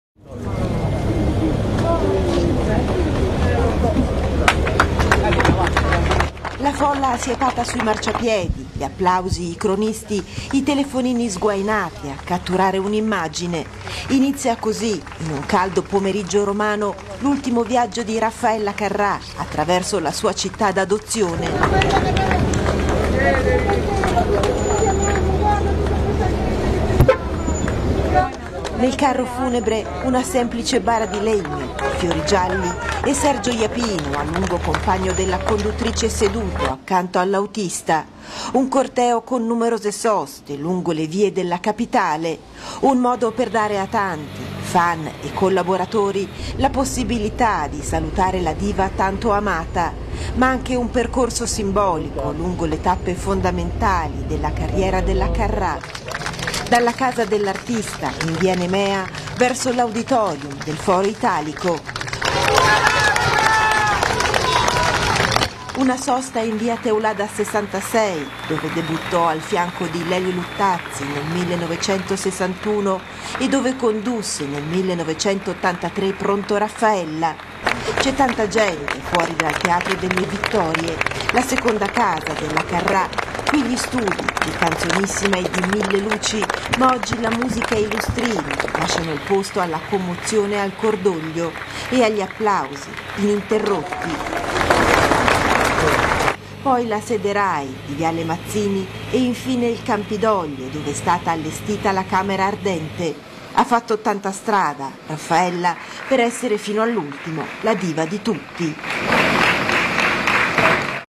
Raffaella Carrà, il lungo addio di Roma
Roma saluta la Raffa nazionale. Un corteo funebre ieri è partito da casa dell'artista per fare tappa nei luoghi simbolo della sua carriera: l'Auditorium Rai del Foro Italico, la Rai di Via Teulada 66, il Teatro delle Vittorie. Per poi arrivare in Campidoglio dove nella Sala Promoteca è stata allestita la camera ardente che resterà aperta fino ai funerali